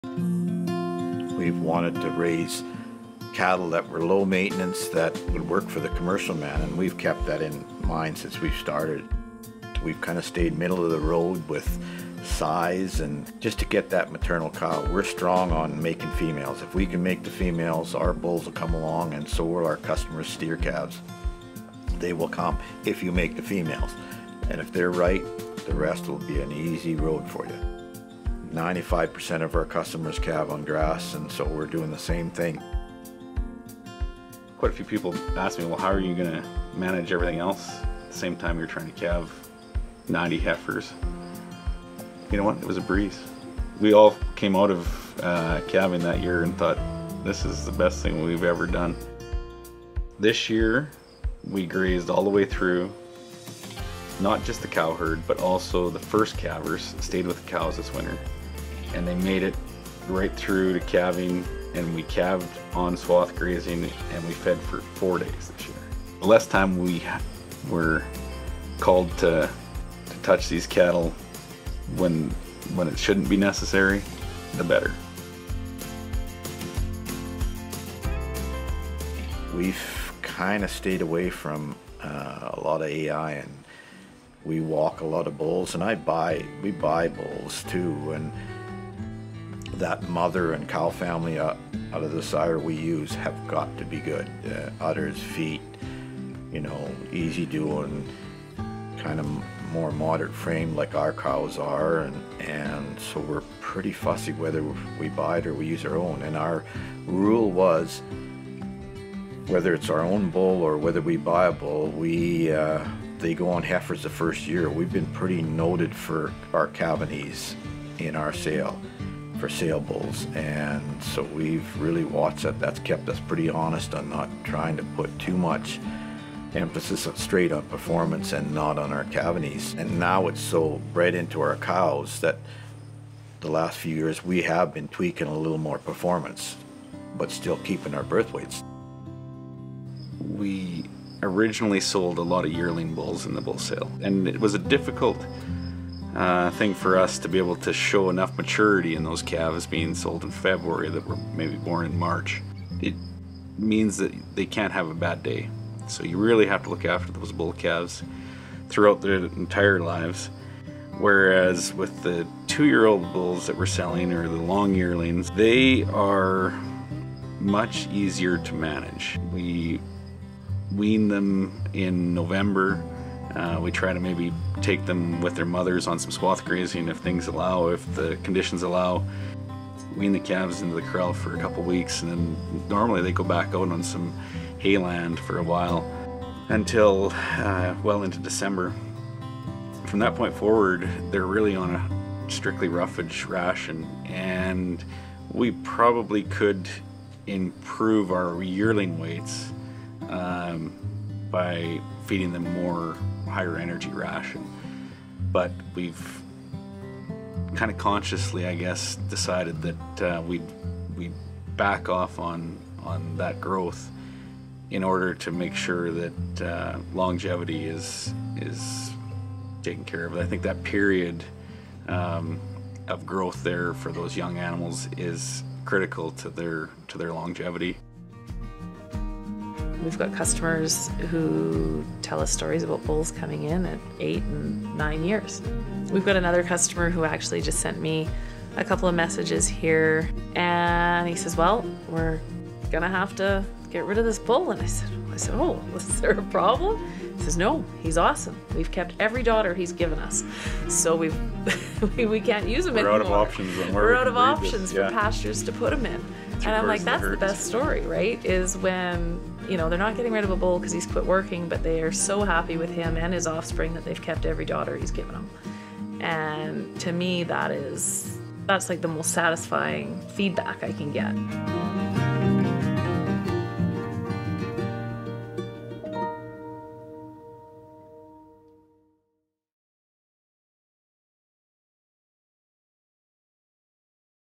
MJT Cattle Company - Our Program
We sat down with Sarah Wray of Storybrokers Productions to bring you some information about our program at MJT Cattle Company.